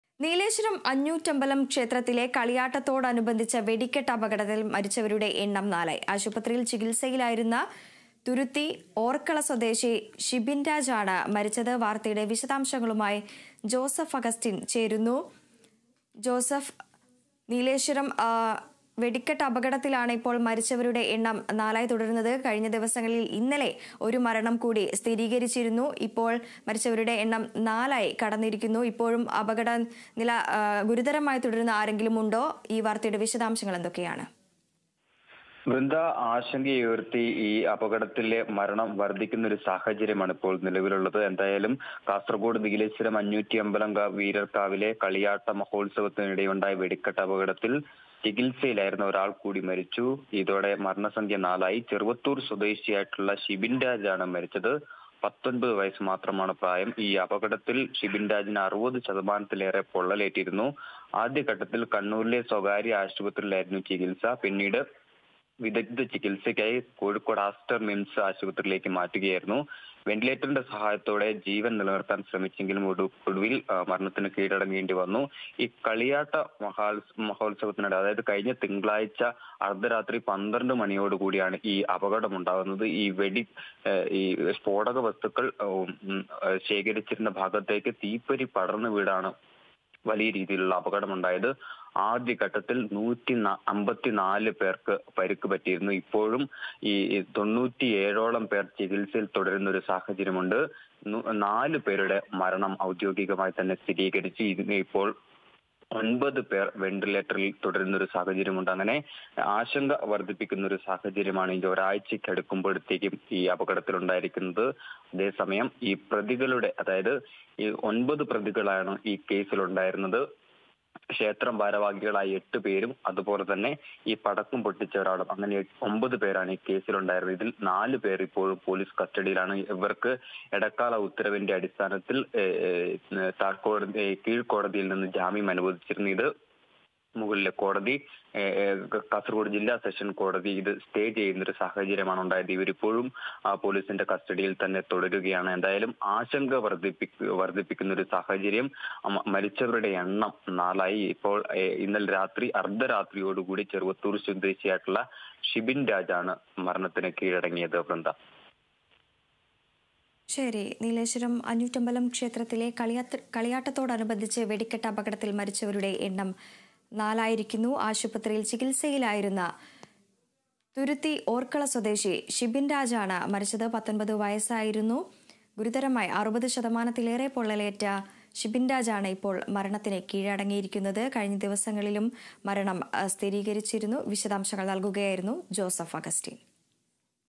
നീലേശ്വരം വെടിക്കെട്ടപകടം; മരണം നാലായി | Kasargode
നീലേശ്വരം അഞ്ഞൂറ്റമ്പലം ക്ഷേത്രത്തിലെ കളിയാട്ടത്തോടനുബന്ധിച്ച വെടിക്കെട്ട് അപകടത്തില്‍ മരിച്ചവരുടെ എണ്ണം നാലായി.  ആശുപത്രിയില്‍ ചികിത്സയിലായിരുന്ന തുരുത്തി ഓര്‍ക്കള സ്വദേശി ശിബിന്‍ രാജ് ആണ് മരിച്ചത്.

About COA